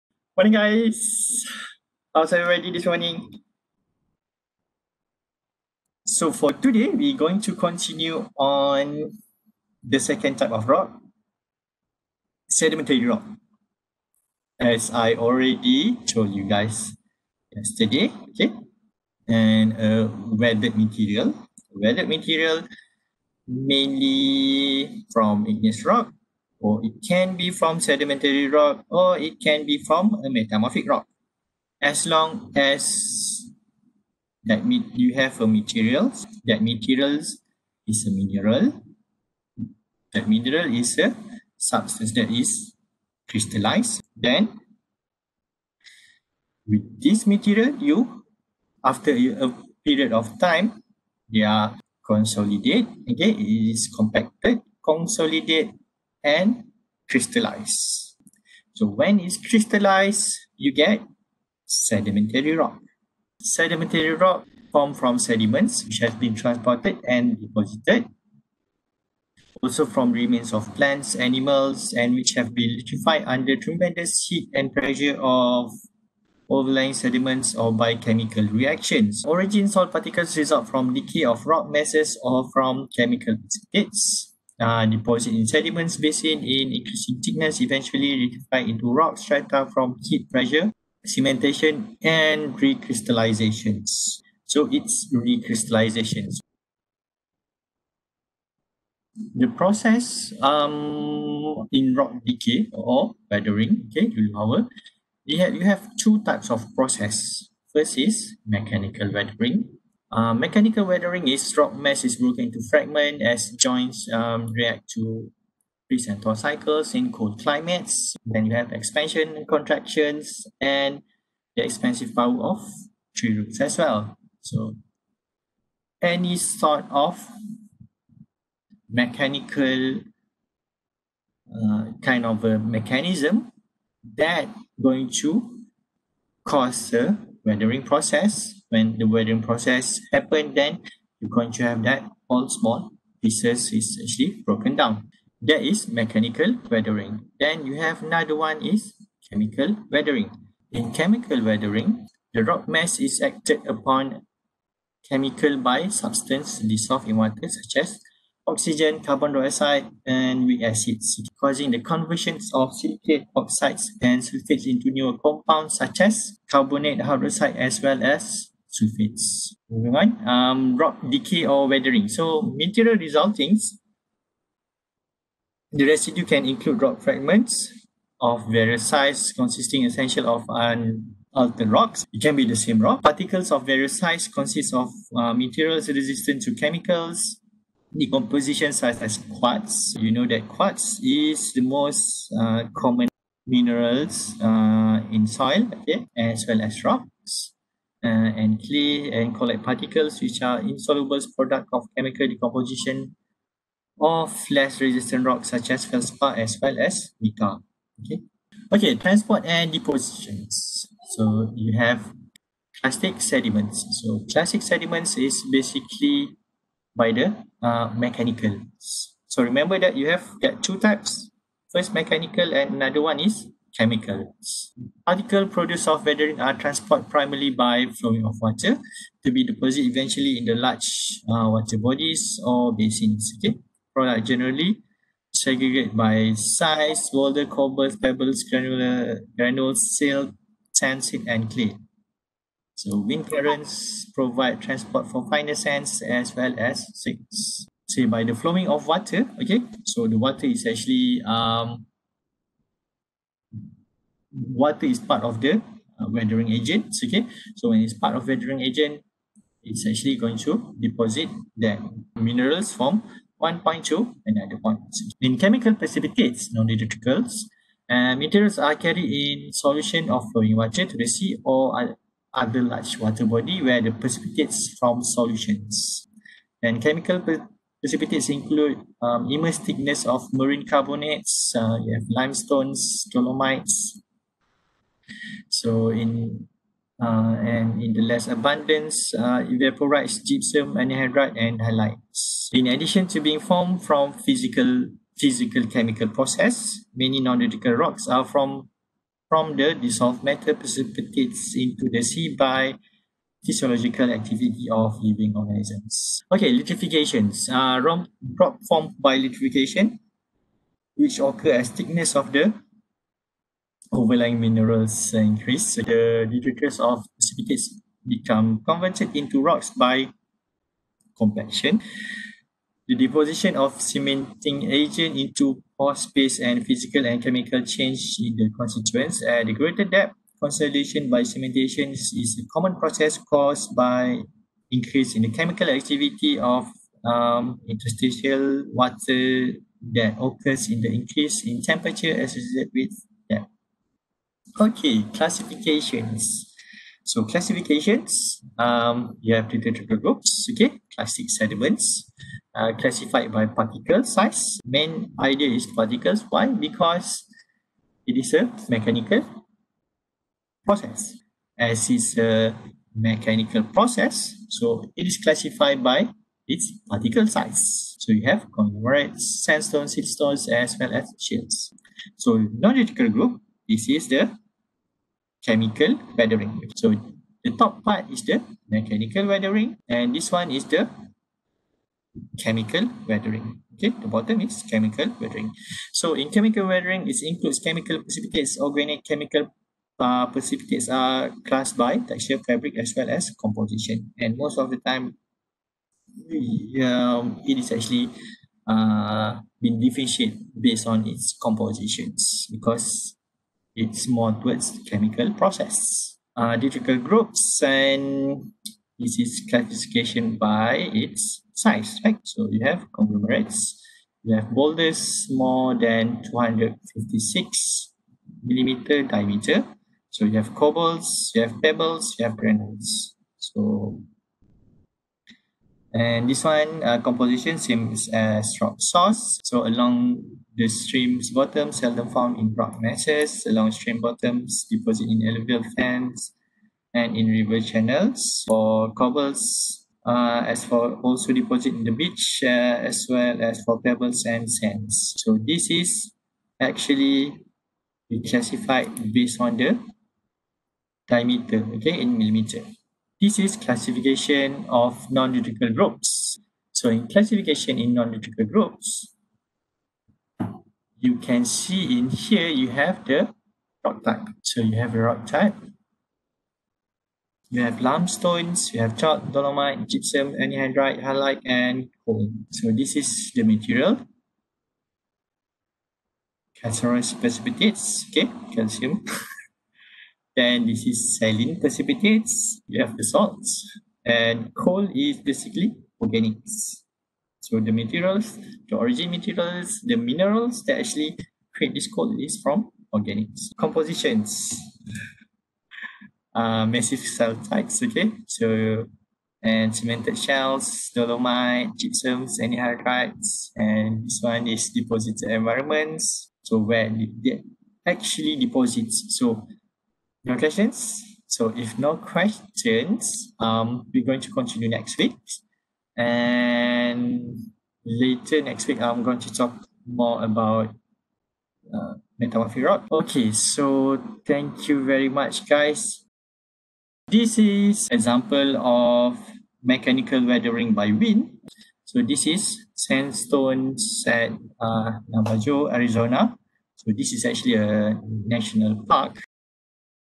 Geology Sedimentary Rock
This is EAG141 lecture on Sedimentary Rock